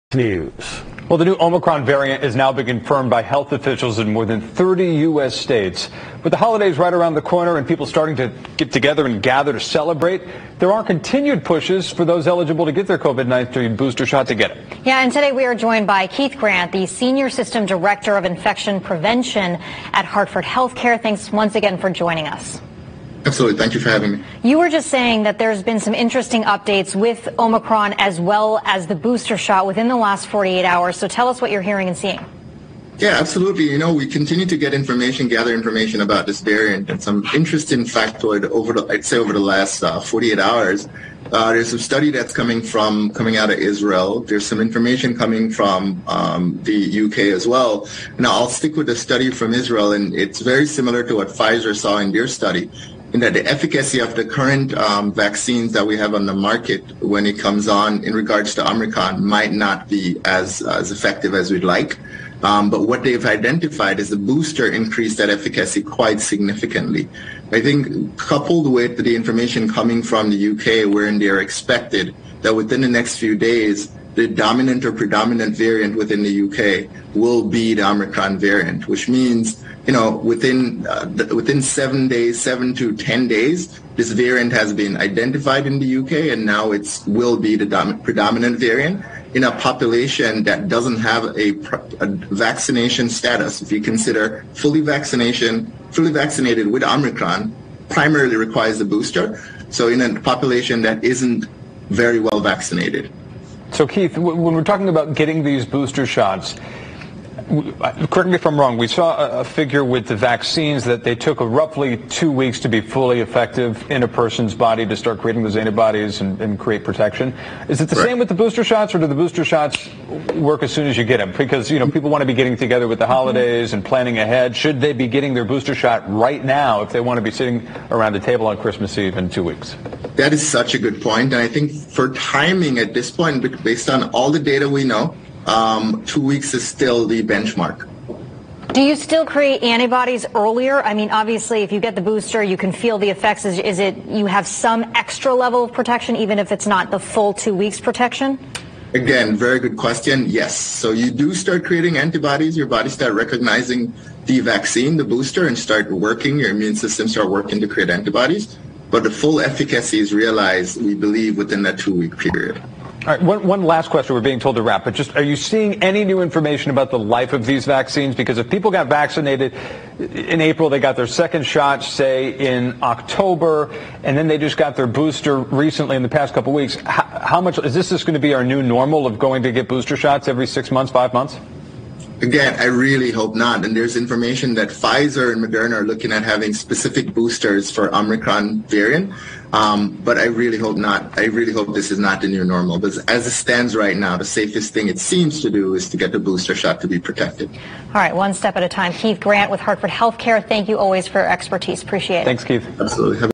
Experts Continue the Push for Boosters
Keith Grant, APRN, Senior System Director, Infection Prevention, Hartford HealthCare, says that a study from Israel has indicated that the current vaccine on the market might not be as effective against Omicron, but that the booster significantly increases that efficacy.